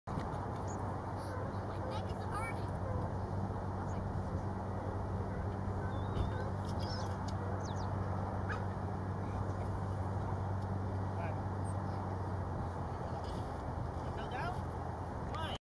White-crowned Sparrow (Salinas River SB)
Immature White-crowned Sparrow (Zonotrichia leucophrys) at Salinas River State Beach, Monterey county, California.  Recorded September 3, 2017.